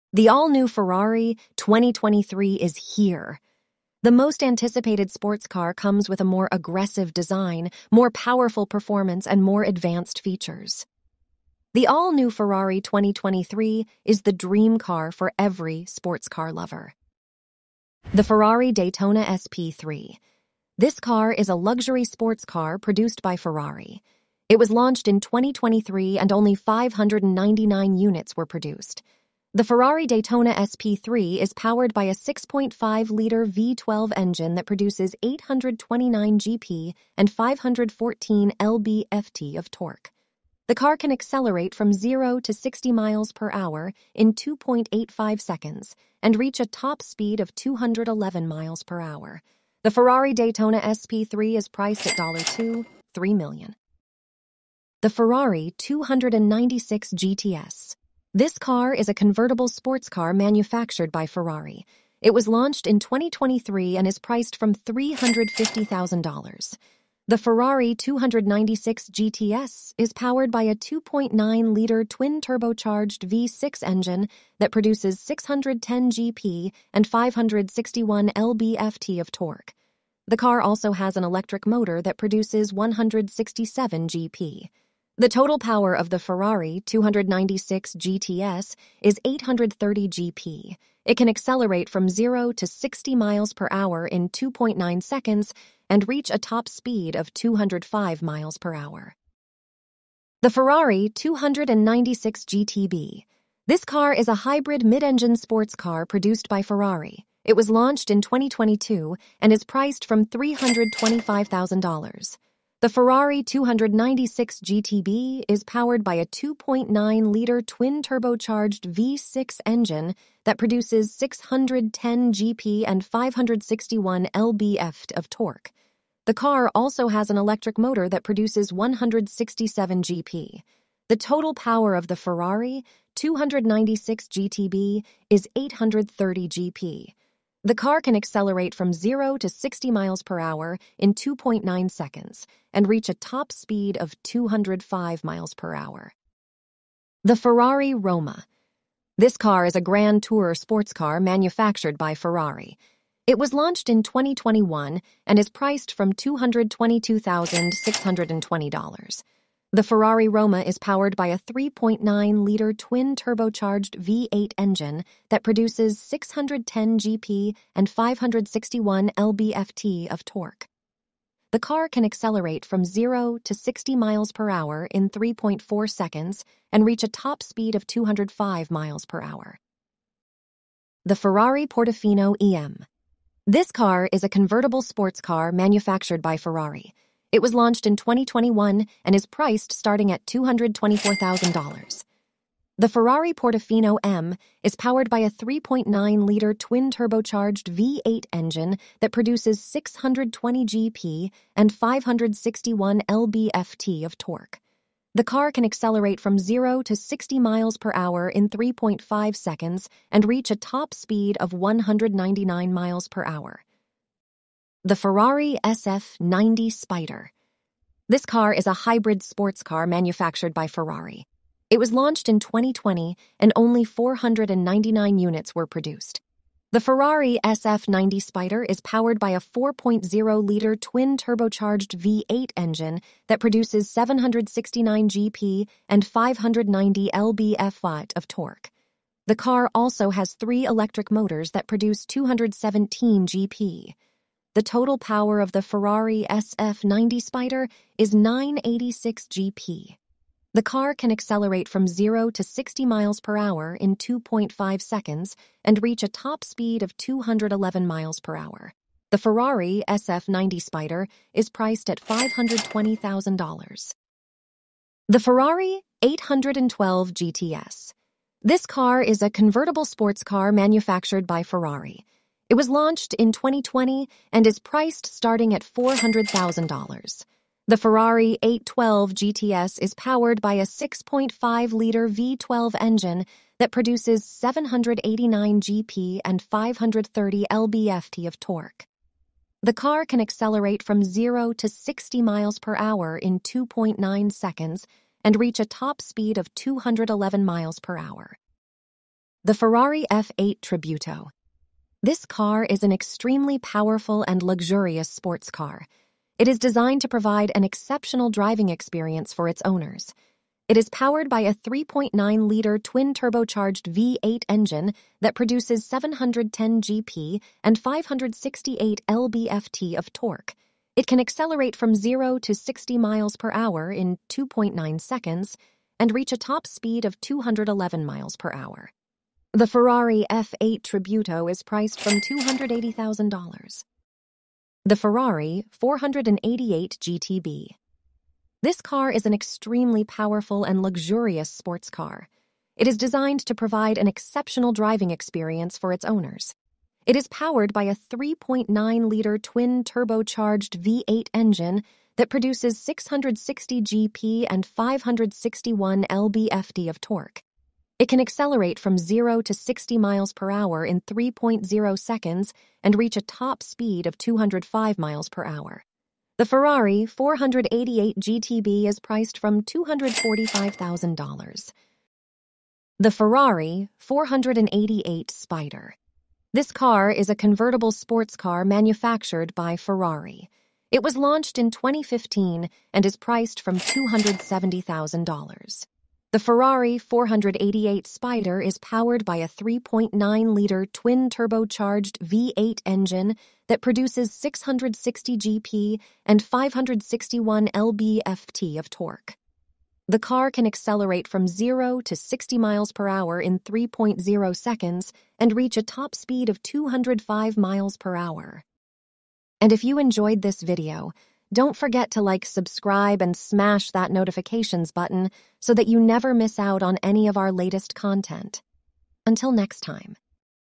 The 2023 Ferrari Revolution: Unveiling the Future of Supercars 🏎️💨
Get ready to be captivated by the beauty and speed that make the latest Ferrari cars an automotive dream come true. What are you waiting for? Join us on this exciting adventure and discover the heart-stopping beauty of the latest Ferrari cars that change the way you think about performance and luxury.

Ferrari Daytona SP3
Ferrari Roma
Ferrari Portofino M
Ferrari SF90 Spider